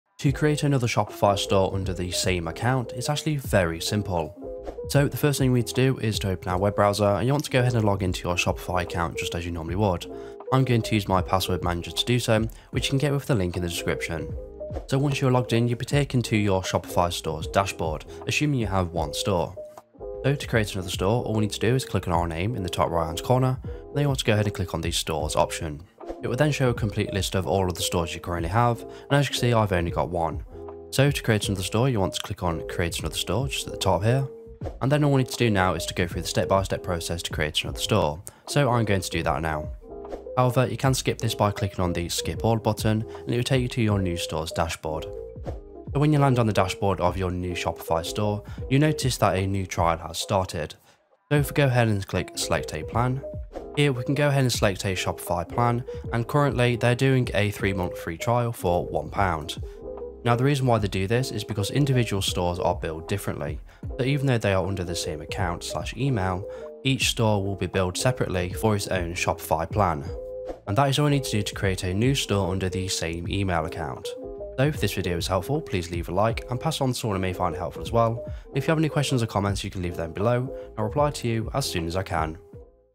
How To Create Another Shopify Store in One Account - (Tutorial)
Today, you will successfully learn how to create another Shopify Store using the same Shopify Account you already have. You can have multiple stores under one account, and each store will be billed individually.